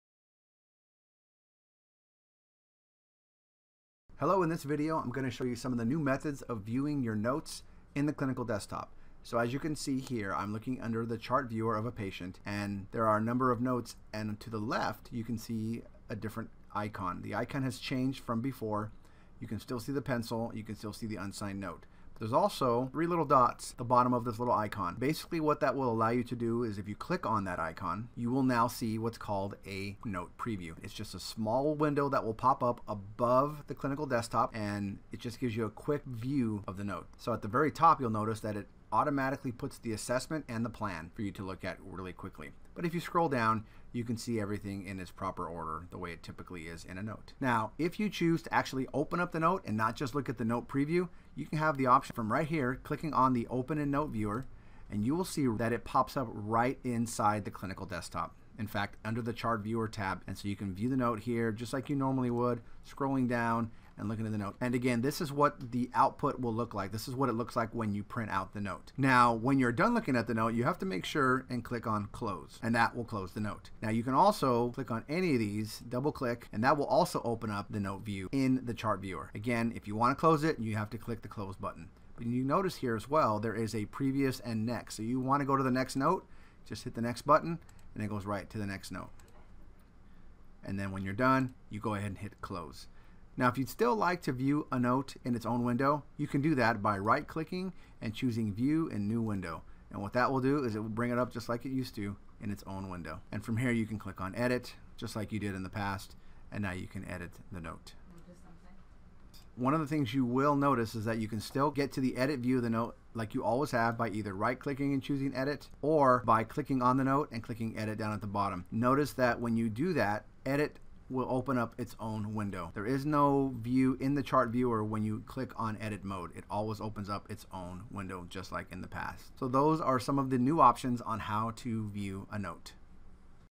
Allscripts EHR 17.1- Note Preview
Video Tutorial on the Note Preview feature in Allscripts Touchworks 17.1